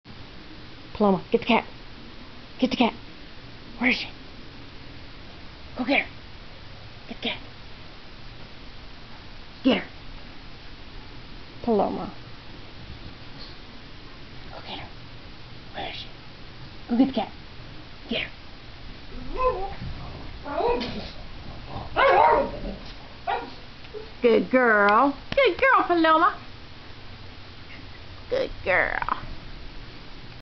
PalomaTheBoston....cat stalker...
Showing off the only circumstance during which Paloma will actually listen to me...Sic the Cat!